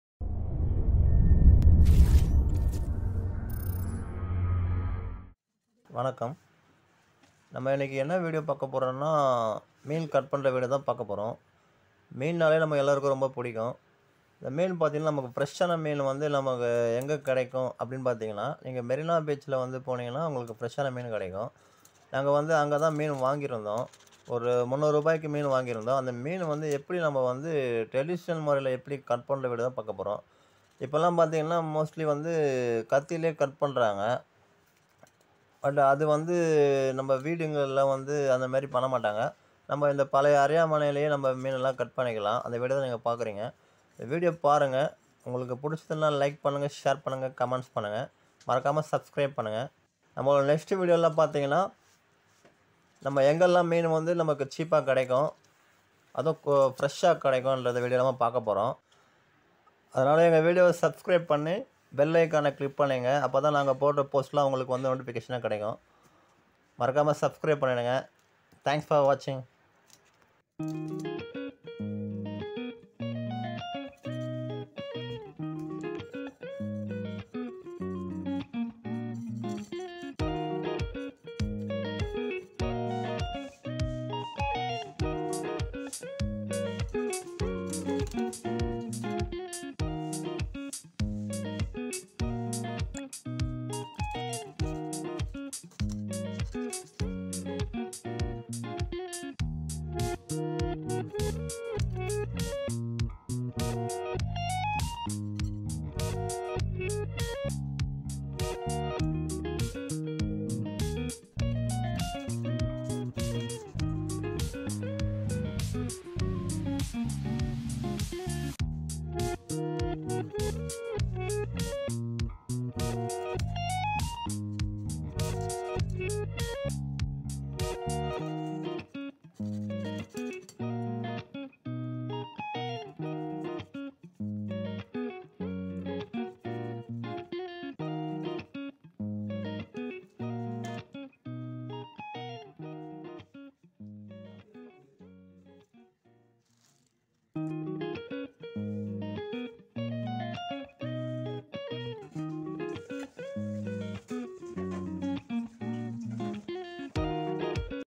fish cutting in traditional way
cheap and fresh fish available at Chennai marina beach